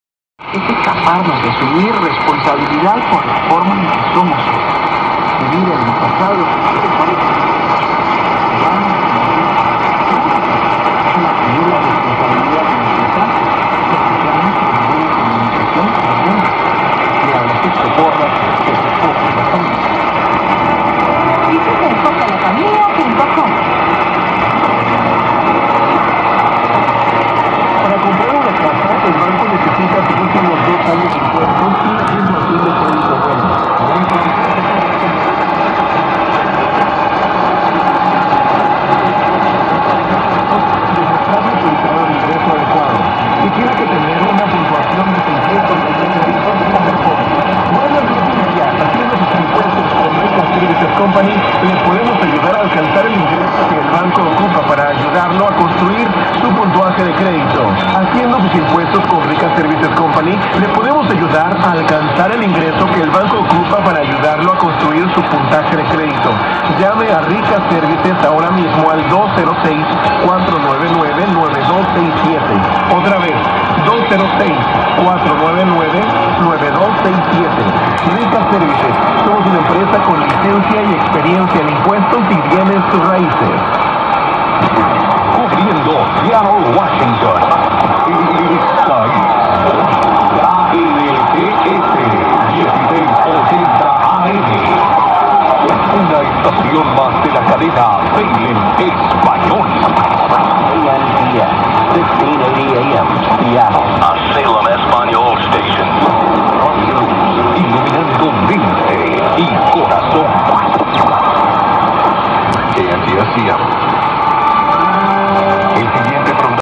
KNTS 1680 Seattle, WA from Sheridan, WY
Received on a stock radio in a 2010 Subaru Impreza. Reception of KNTS 1680 in Seattle, WA USA from Sheridan, Wyoming. This video was shot on February 17th, 2015. The top of the hour ID starts at 1:26 into the video.